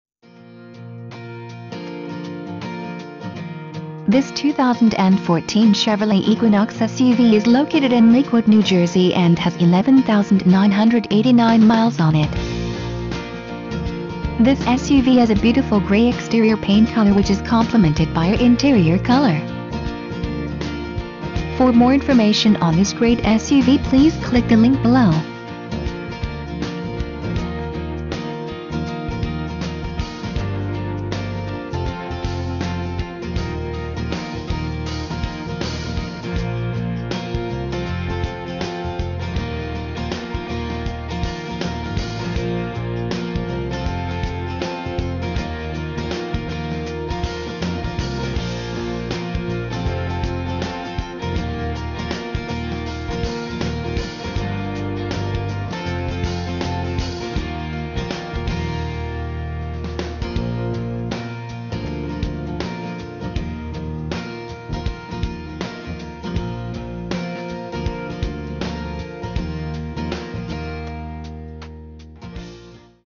2014 Chevrolet Equinox Lakewood NJ
Price: $ 23,608
Mileage: 11,989
Trim: 1LT
Body Style: SUV
Ext. Color: Gray
Int. Color:
Engine: 2.4L I4 16V GDI DOHC
Drive Type: Front-Wheel Drive
Fuel Type:
Doors: 4
Stock No.: L215286
VIN: 2GNALBEK0E6215286